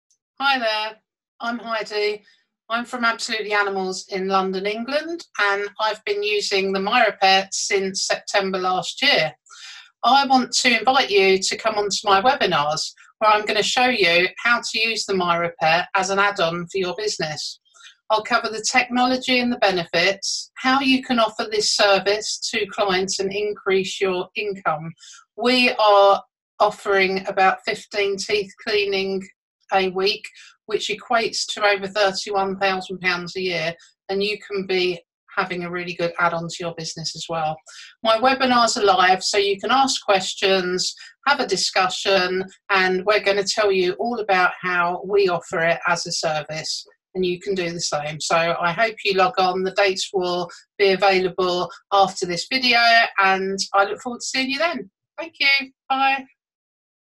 How to boost your income: Free Webinar with Super-Groomer Heidi Anderton
Learn from Super-Groomer Heidi Anderton (owner of Absolute Animals in London in England, the most respected and recognised grooming school in all of Great Britain) how to use Mira-Pet and how to grow your business.

Heidi Anderton demonstrates how to use the Mira-Pet ultrasound dog toothbrush on pets and she also explains how a typical single groomer can make an extra 35,000 USD (31,000 British Pounds) per year in extra income from dental service for pets.

✔ 80% of all dogs over the age of 3 years suffer from gum inflammation or periodontitis.
✔89.7 million dogs live in US households. This is a huge market and opportunity for Groomers to boost vour income by offering dental cleaning services.
✔ Many dog owners are searching for smart teeth cleaning services on a regular basis.
✔Professional vet treatment is very expensive for the dog owners. And it requires anesthesia. Whereas you can offer anesthesia-free treatment.